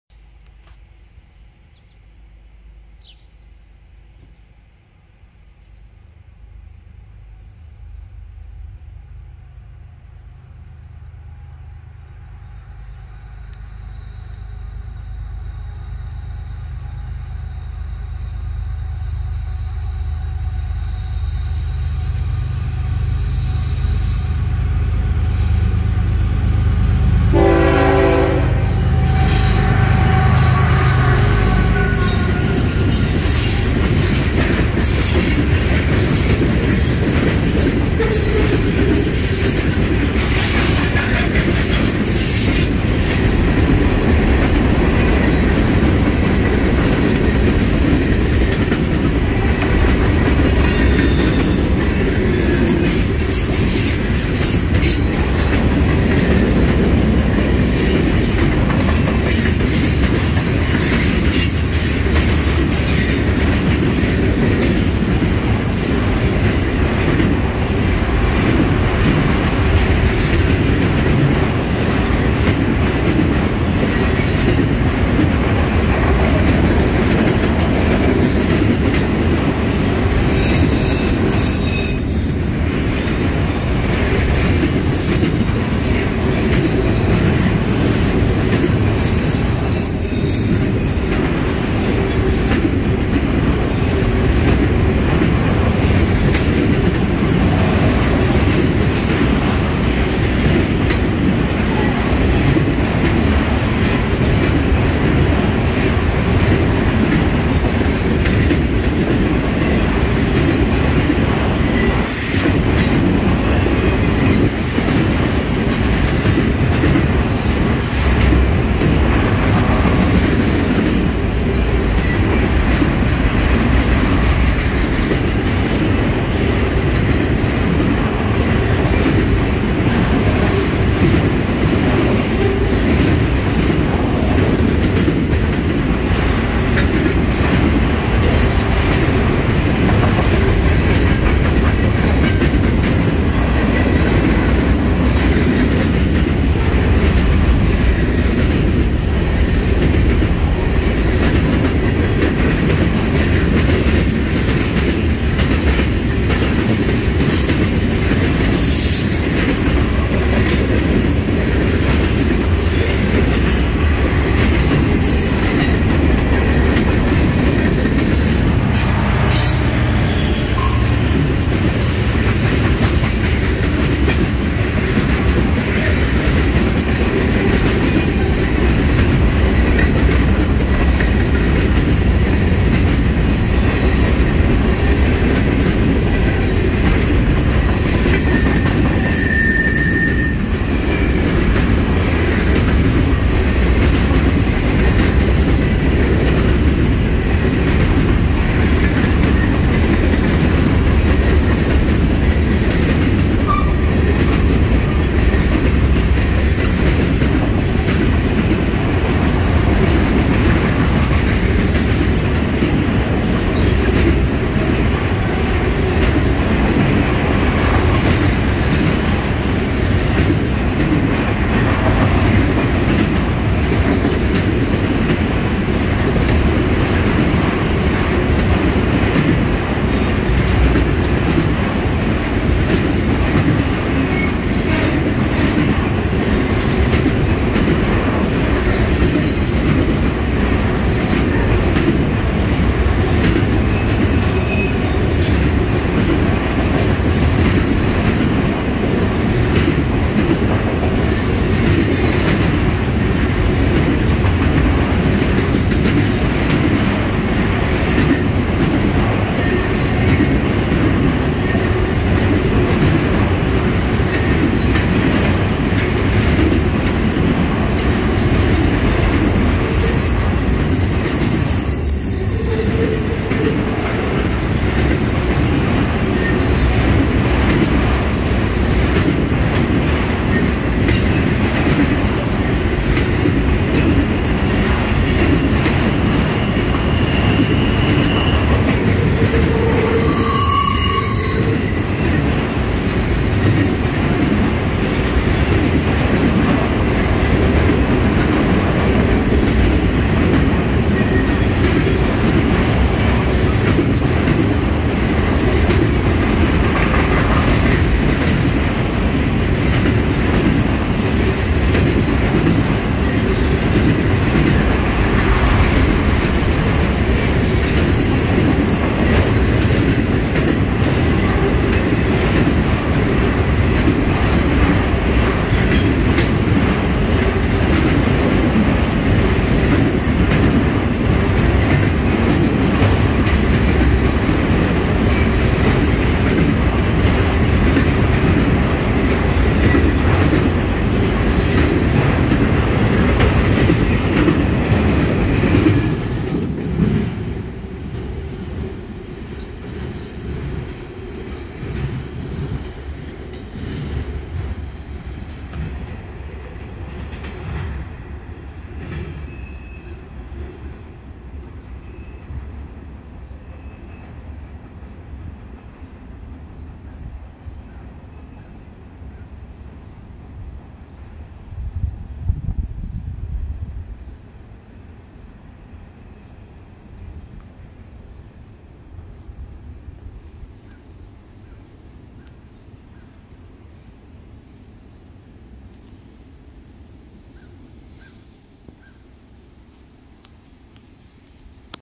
Norfolk Southern 8164 leads a 178 car manifest at Lewistown, PA (07/19/2022)
13V (second edition; featuring large quantities of autoracks)
If you like my videos, consider joining this channel to get access to perks: